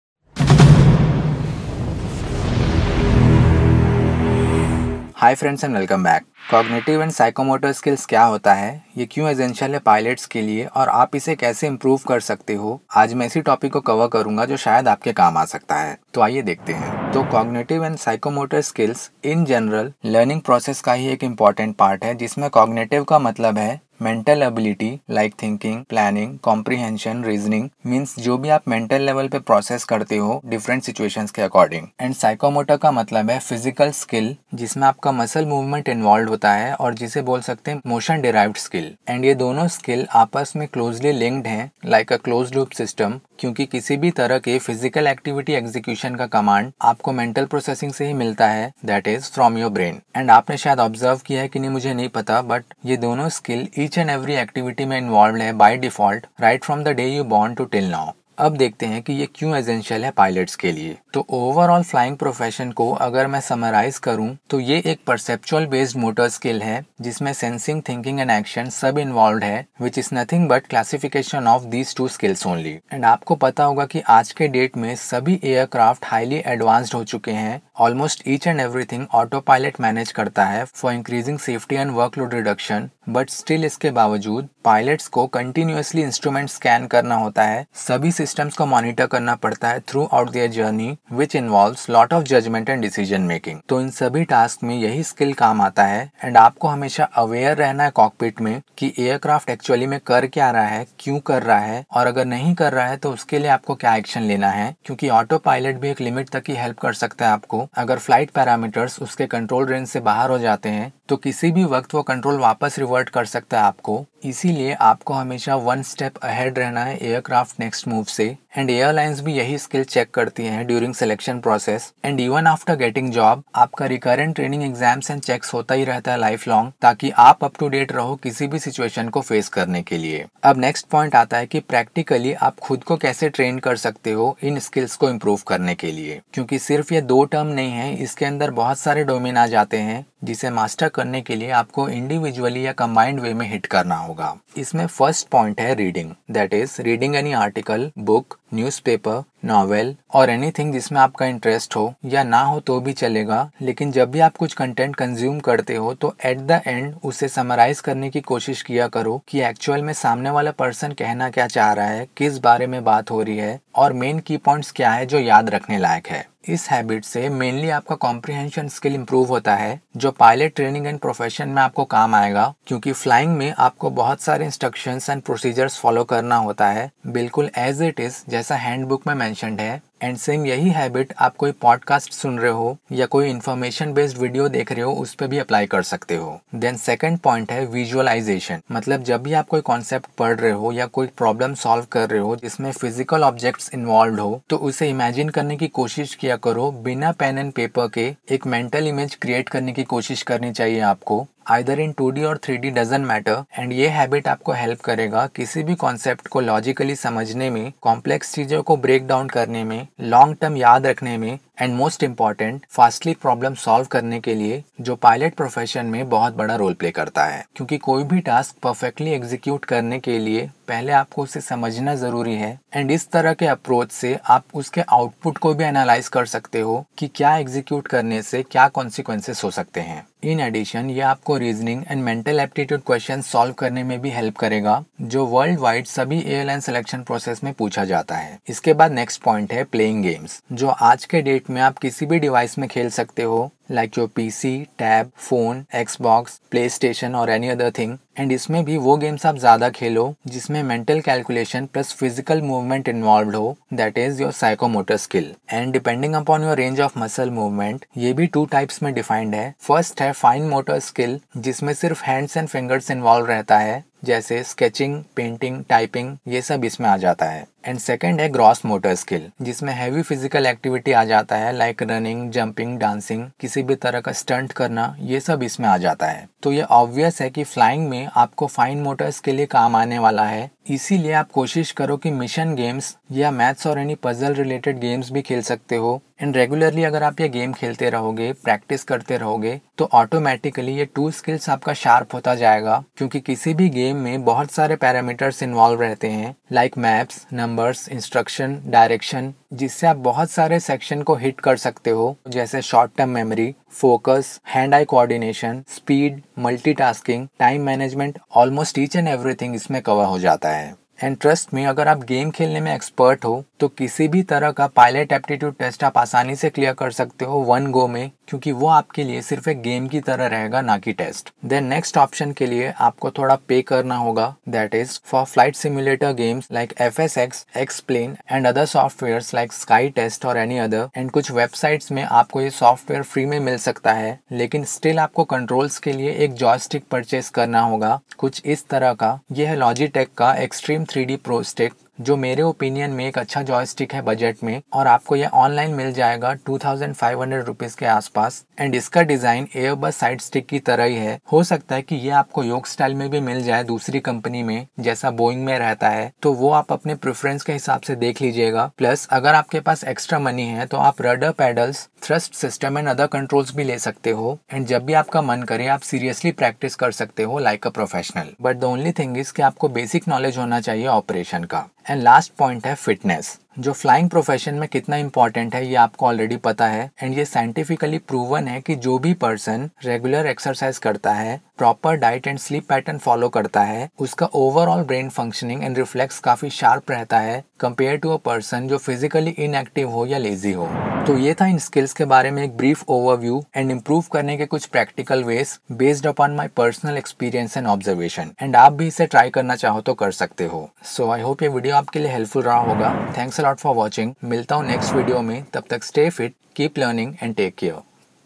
Why Cognitive & Psychomotor Skill Essential For Pilots | How To Improve It 💁🏻‍♂️
In this video i have briefly covered about cognitive and psychomotor skill which is a part of learning process in general but very essential for pilot profession because its a perceptual based motor skill which requires lot of judgment, analysis and decision making throughout, so if you want to become a good pilot or planning for it, then these skills has to be mastered because its also checked during airline selection process, specifically in pilot aptitude test, i have also covered some practical ways to enhance it based upon my personal experience and observation so far, which you can also try if you want. So i hope you like this video and it helps you in someway. Thanks

Thanks & Regards
Cadet Pilot